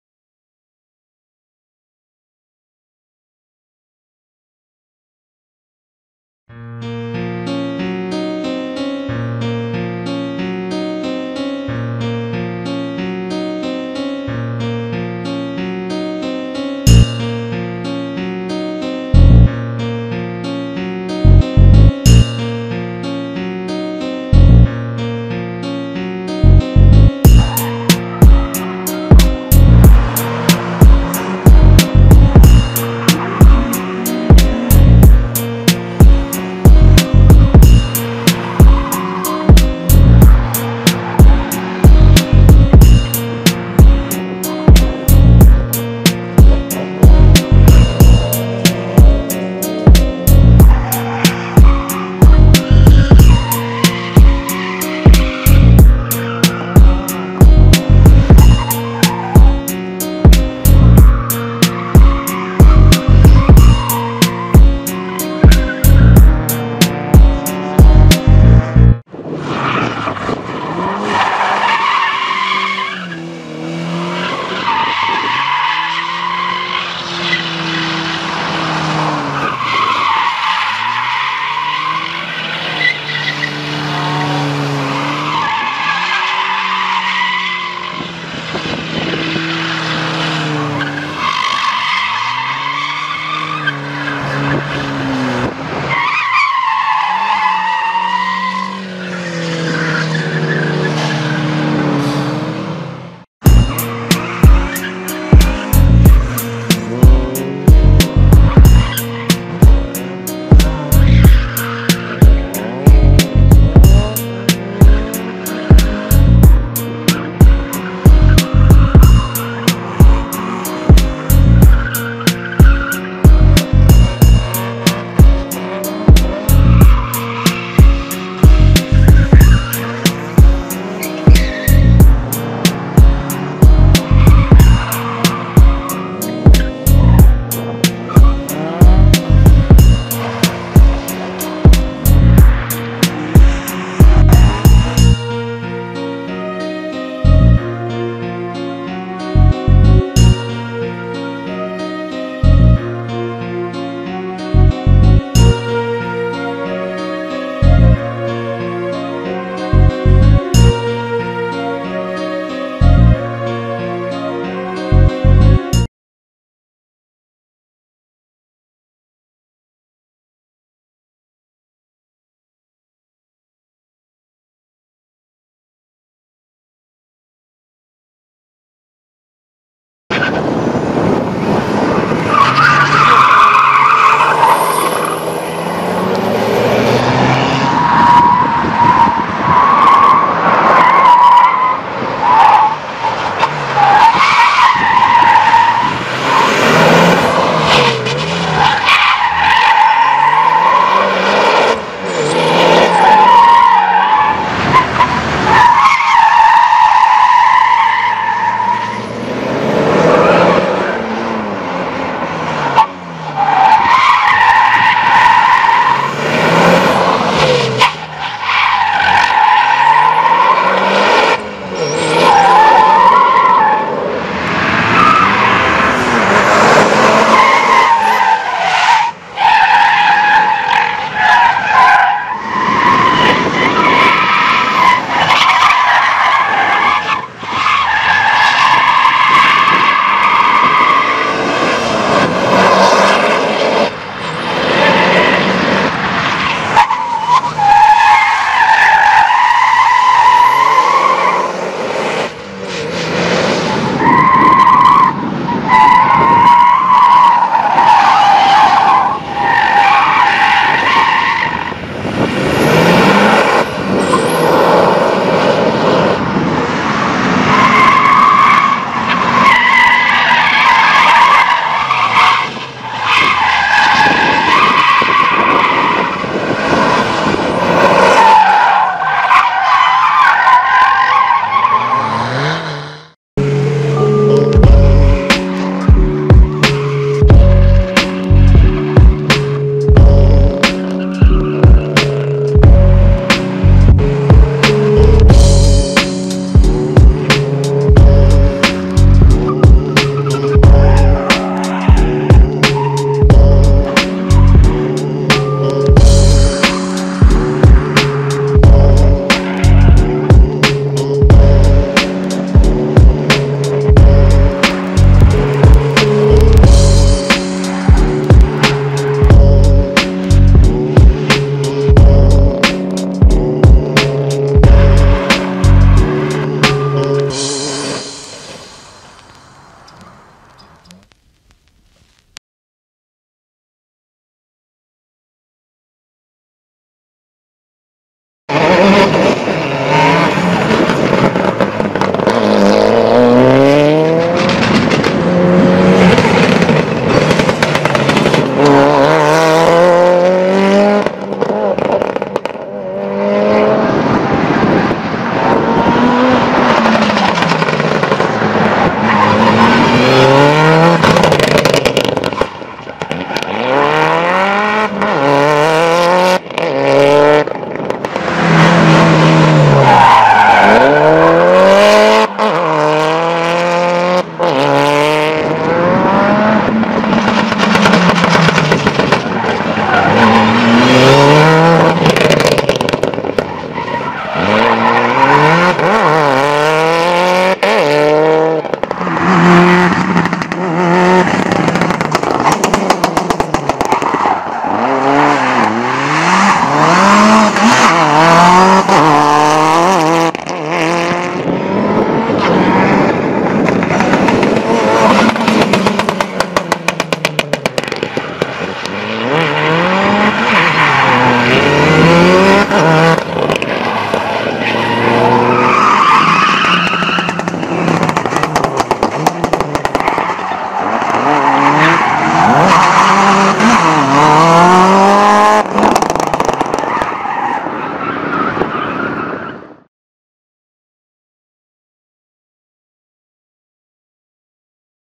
Automobilový slalom Vyškov 10.11.2018
Na posledním Autoslalomu Vyškov s rekordní jezdeckou účastí (81) jsem pořídil hodně záběrů . Do prvního videa jsem jich však zařadil jen pár. Protože ostatní záběry mi bylo líto smazat, rozhodl jsem se vytvořit video druhé.

1:09 - Bláznivý Ford KA
2:46 - Největší hvězda, která kdy na slalomu ve Vyškově startovala
5:33 - Vítěz Autoslalomu Vyškov Lukáš Štelc